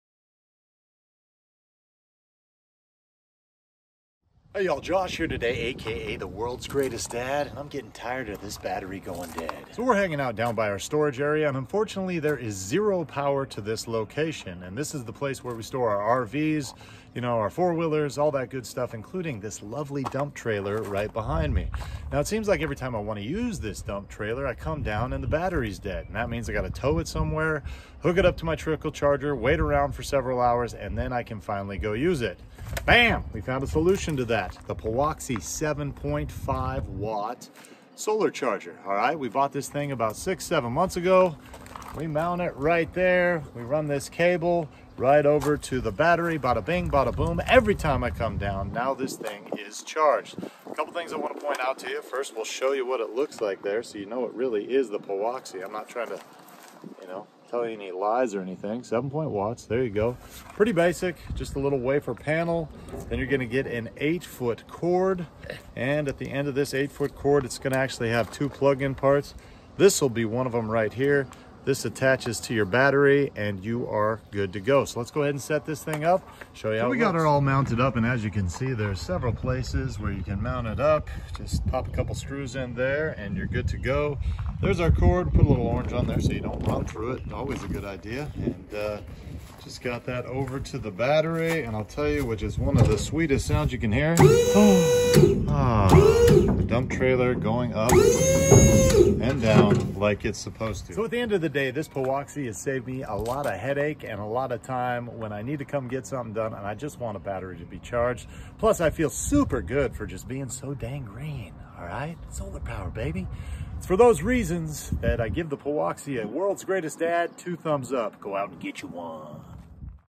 Does POWOXI Make the Best Budget Solar Trickle Charger on Amazon?
🔥 Go Out & Getcha One!

Looking for the best solar powered trickle battery charger for your car, truck, motorcycle or boat? The POWOXI Upgraded 7.5W Solar Battery Trickle Charger & Maintainer with a 12V Portable Waterproof Solar Panel might be exactly what your looking for.

In today's video the World's Greatest Dad provides an in-depth review on the POWOXI Upgraded 7.5W Solar Battery Trickle Charger & Maintainer. Follow along as I not only demo the product but also tell you everything else you need to know about this sweet little solar trickle charger as well.

More information & features of the Powoxi solar powered trickle charger:

✅ Comprehensive Battery Protection
This solar car battery trickle charger fully protects the battery thanks to the built-in blocking diode structure that ensures no reverse discharge, overcharge, overvoltage, or short circuit.

✅ Upgraded Intelligent Charger Controller
This solar car battery charger generates at least 25% - 30%more power than the other brand solar panels, and it even can charge your battery on cloudy days.

✅ Stronger and More Durable
The Powoxi solar car charger adopts high-transparency glass, which is high-strength, impact-resistant, wind-snow-resistant low-iron tempered. And the sturdy ABS frame makes the solar panel more durable than others.

✅ Easy to Use & Install
The solar car battery maintainer is easy to install and convenient to use. Simply connect the solar panel to the battery with the alligator clips and you are good to go. It's a versatile charger / maintainer for any car, truck, motorcycle, boat, tractor, ATV, farm equipment, and LiFePO₄, Lithium Ion batteries.

✅ Warranty Included
The Powoxi Solar charger comes with a 12-month warranty with lifetime technical support as well.

So... Is this the best solar powered trickle battery charger for your vehicle? While there are certainly a lot of options out there, this solar charger has performed extemely well for me on multiple vehicles. It's for that reason and more that I easily give the Powoxi solar powered trickle charger a World's Greatest Dad thumbs up and I say...